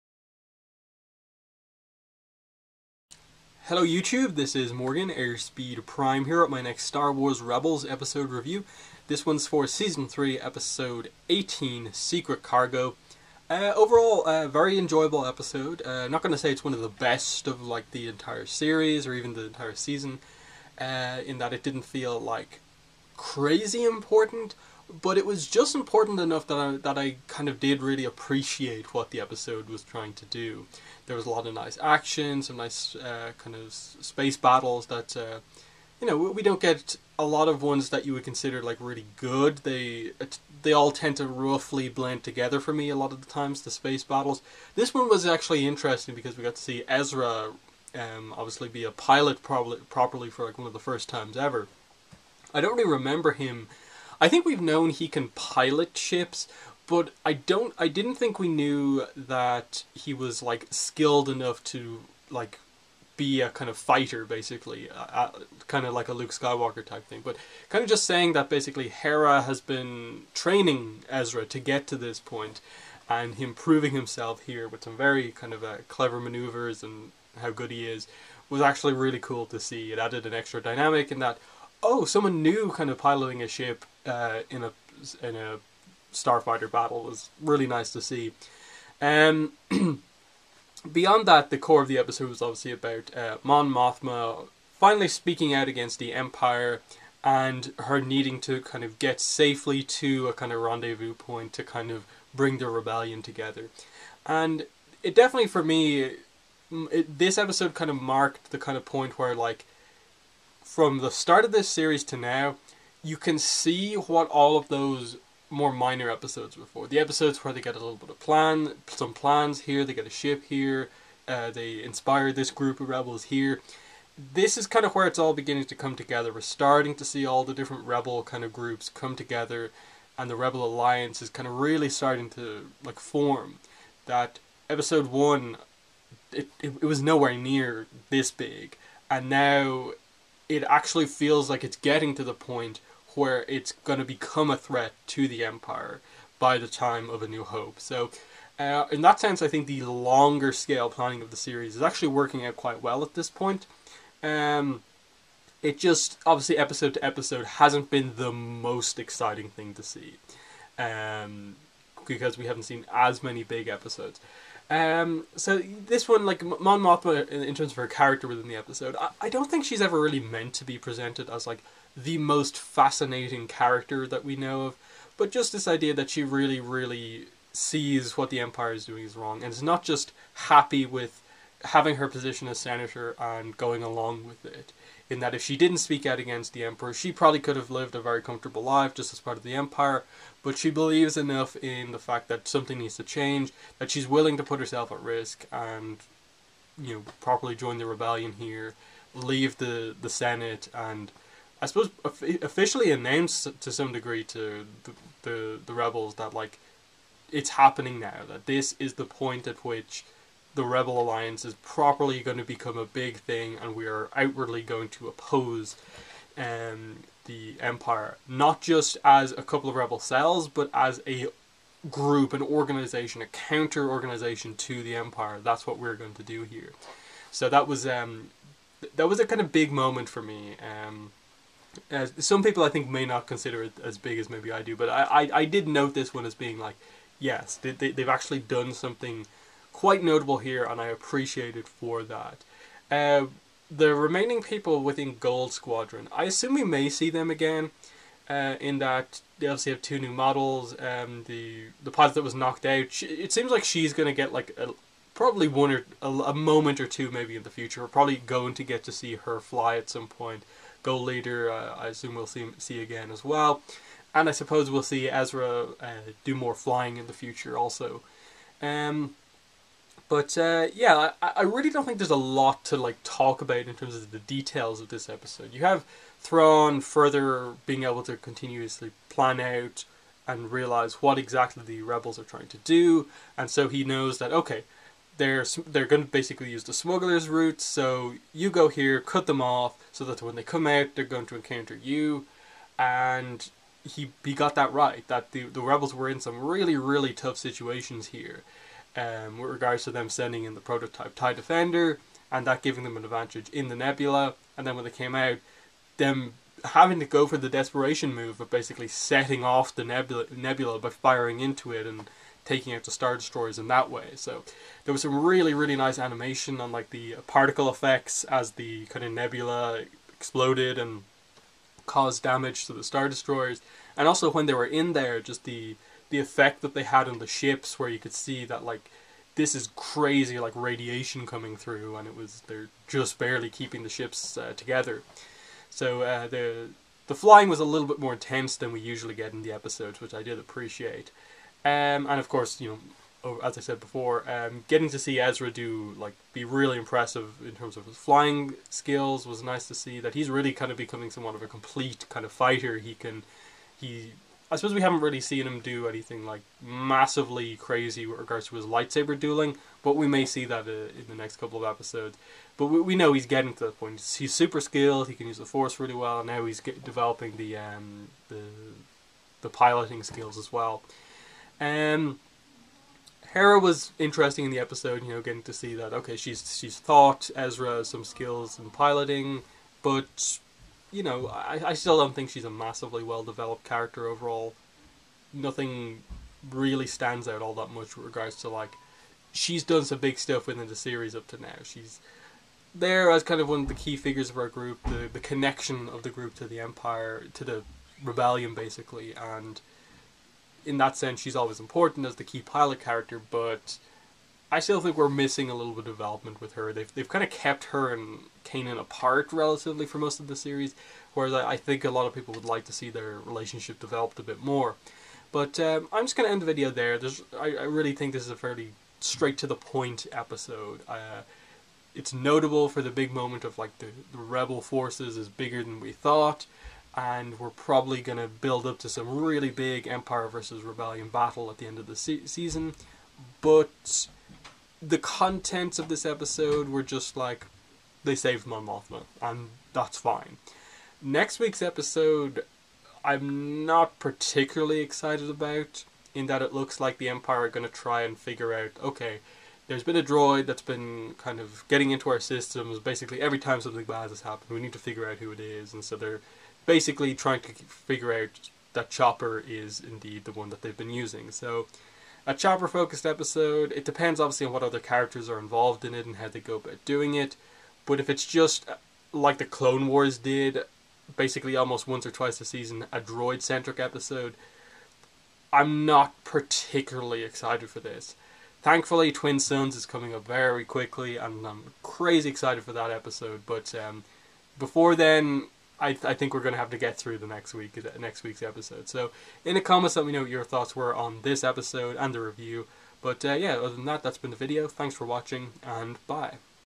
Star Wars Rebels Episode Review - 318 Secret Cargo
I review the latest episode of Star Wars Rebels, Season 3 Episode 18 Secret Cargo.